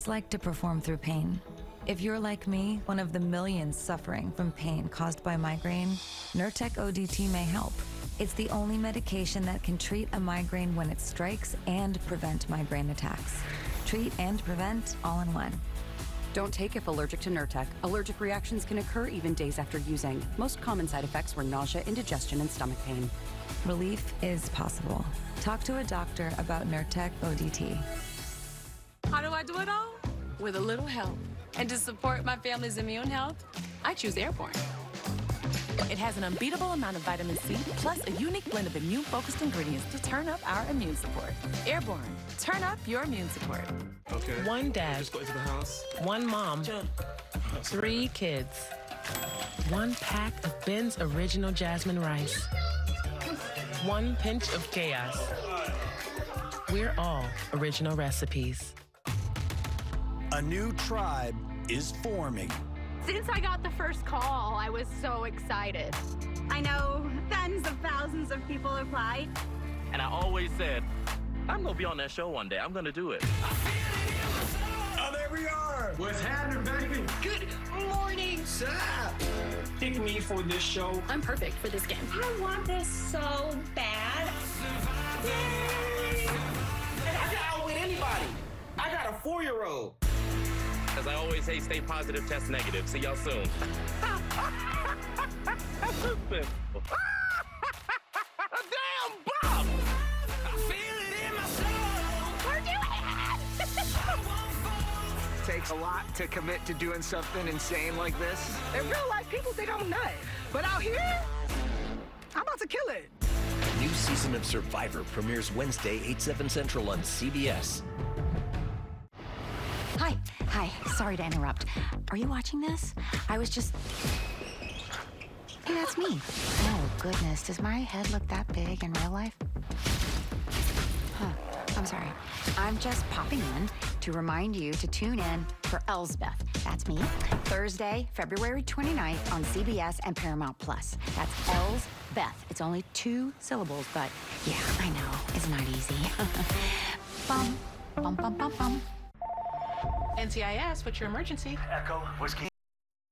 TV Commercials Nurtec Ft. Lady Gaga Airborne Ft. Kelly Rowland Uncle Bens
Welcome to a collection of TV commercials featuring Nurtec featuring Lady Gaga, Airborne featuring Kelly Rowland, and Uncle Ben's! 📺✨

In this video, prepare to be entertained and inspired by these dynamic advertisements from some of today's most recognizable brands.

First, join Lady Gaga as she shares her experience with Nurtec, offering relief for migraine sufferers everywhere.

Then, get ready to boost your immune system with Airborne, featuring Kelly Rowland, as she highlights the benefits of staying healthy and vibrant.

And don't miss out on the comforting and delicious meals from Uncle Ben's, providing convenient and tasty options for every occasion.

With star power and quality products, these TV commercials are sure to leave a lasting impression.

Recorded 02/26/24 in Detroit, Michigan USA